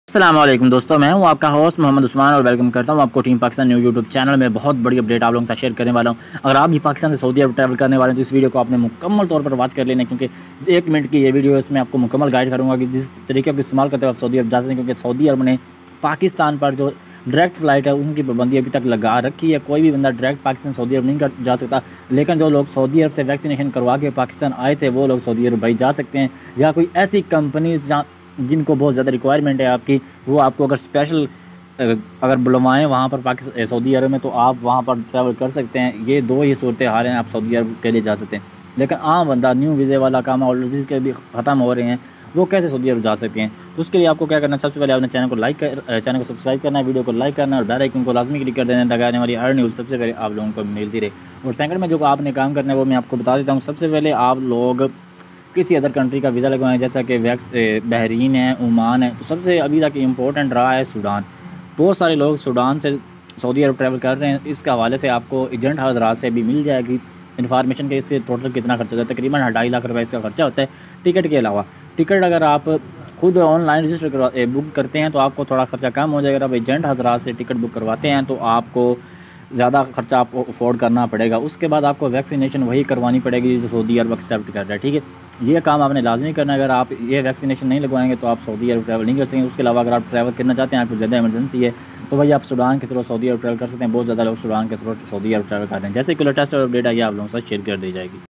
Pakistan To Saudi Arabia Flight Open | Saudi Ministry of Interior Announce Flights Start Pakistan
Hello viewers today i am telling you about Pakistan To Saudi Arabia Flight Open | Saudi Ministry of Interior Announce Flights Start Pakistan

pakistan To Saudi Arabia Flight Open | GACA Announce Flight Start 28 For Pakistan